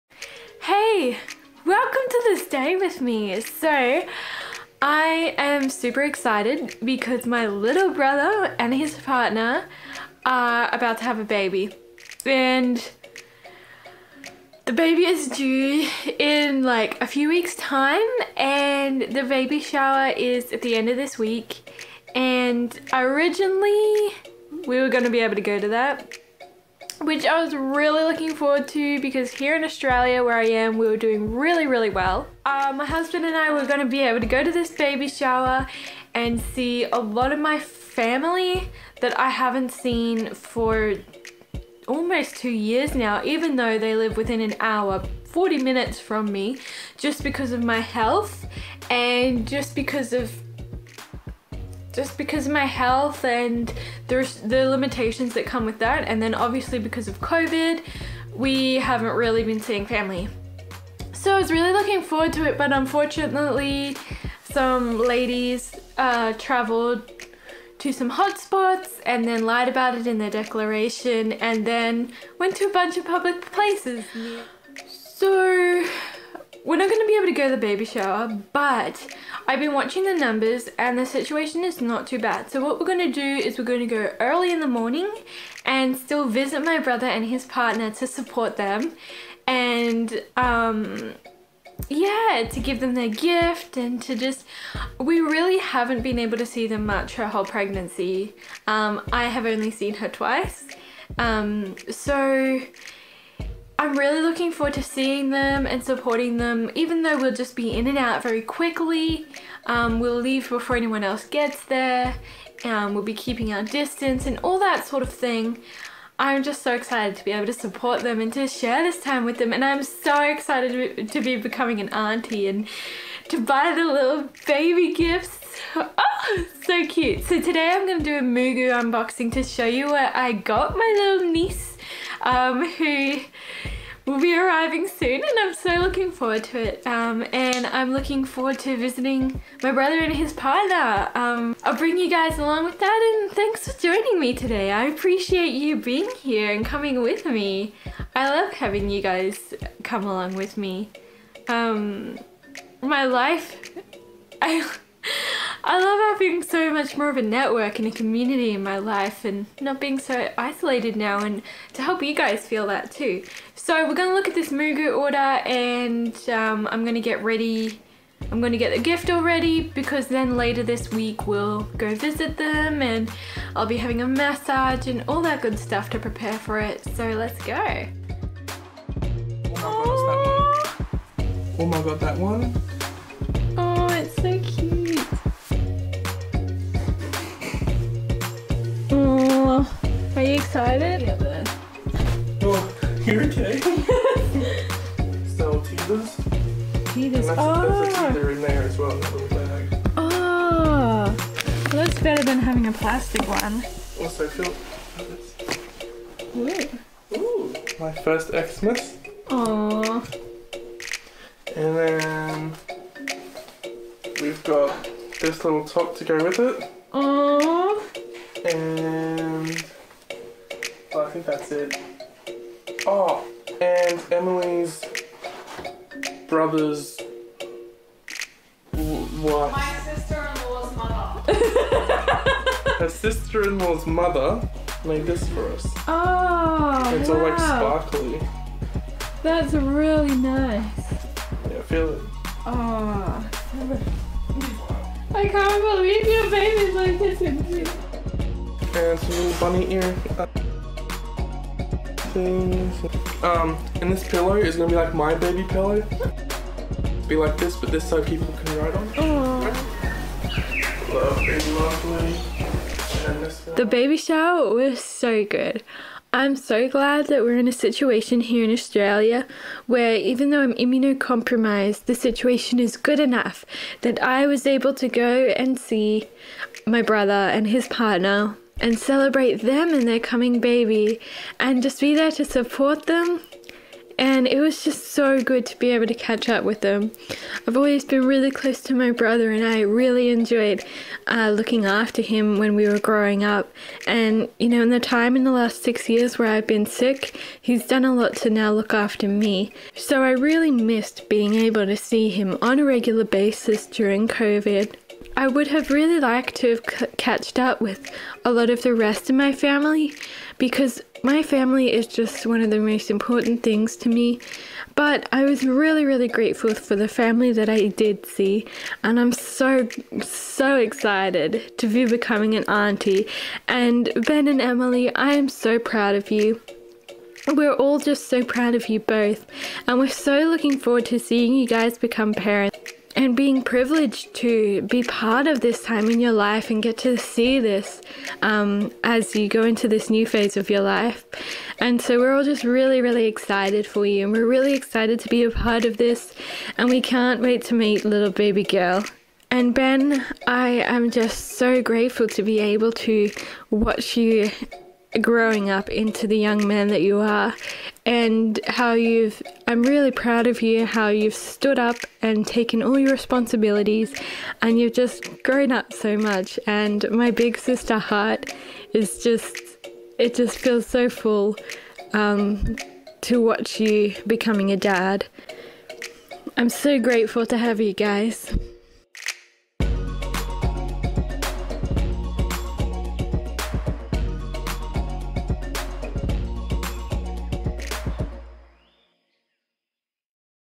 My Little Brother is about to have a BABY!!! (filmed months b4 uploading)
Even though this was filmed several months before uploading, I thought some of you may still enjoy the video :) Simeon and I were about to visit my brother and his partner before their baby shower and it was such a sweet time getting to see all their preparations taking place. This is one of my first non-medical videos if you would like to get to know more about me :)